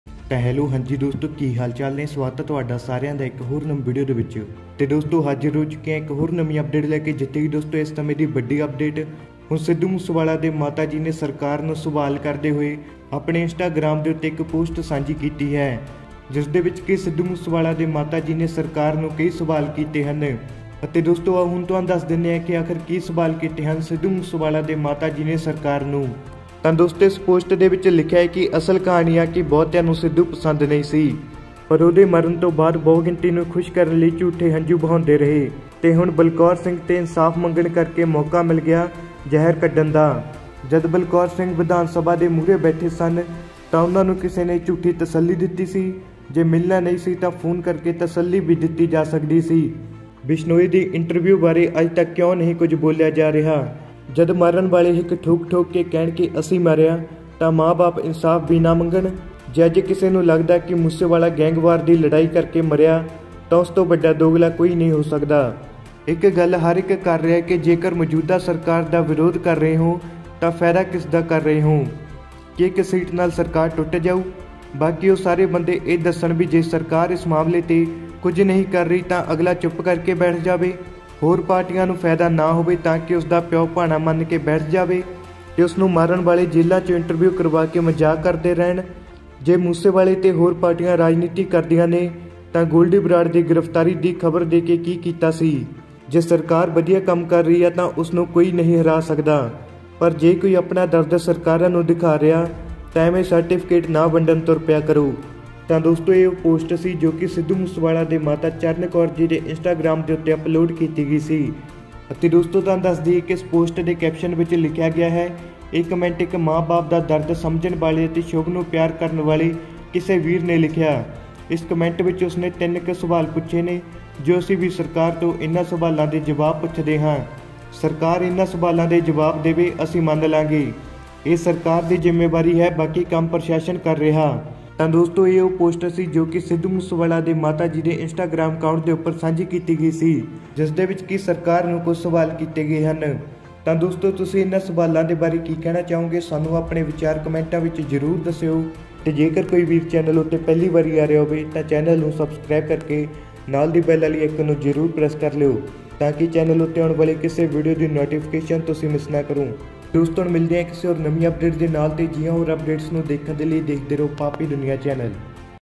Sidhu Moosewala Mother Instagram Post | Sidhu Moosewala Mother Ask Questions To Government
Paapi Dunia